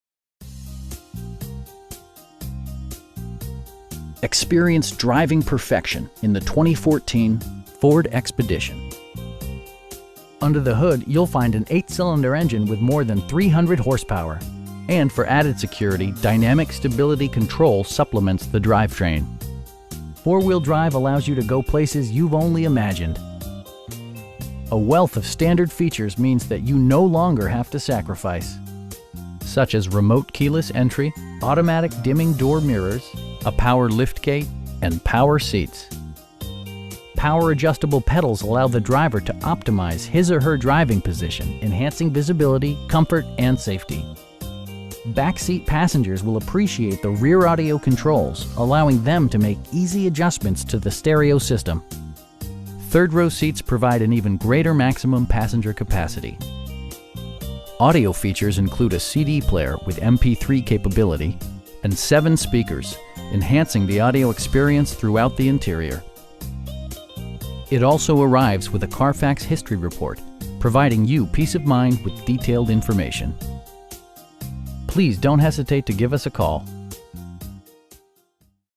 2014 Ford Expedition 4WD 4dr Limited
Experience driving perfection in the 2014 Ford Expedition. Under the hood you'll find an 8 cylinder engine with more than 300 horsepower, and for added security, dynamic Stability Control supplements the drivetrain. Four wheel drive allows you to go places you've only imagined. A wealth of standard features means that you no longer have to sacrifice. Such as remote keyless entry, speed sensitive wipers, a built-in garage door transmitter, automatic dimming door mirrors, front bucket seats, a power liftgate, power windows, and power seats. Features such as automatic climate control and leather upholstery prove that economical transportation does not need to be sparsely equipped. Power adjustable pedals allow the driver to optimize his or her driving position, enhancing visibility, comfort and safety. Back seat passengers will appreciate the rear audio controls, allowing them to make easy adjustments to the stereo system. Rear passengers enjoy the seat heating functionality, keeping them warm during the winter months. Third row seats provide an even greater maximum passenger capacity. Audio features include a CD player with MP3 capability, steering wheel mounted audio controls, and 7 speakers, providing excellent sound throughout the cabin. Safety equipment has been integrated throughout, including: dual front impact airbags with occupant sensing airbag, front side impact airbags, traction control, brake assist, a security system, and 4 wheel disc brakes with ABS. It also arrives with a Carfax history report, providing you peace of mind with detailed information. We pride ourselves in consistently exceeding our customer's expectations. Please don't hesitate to give us a call.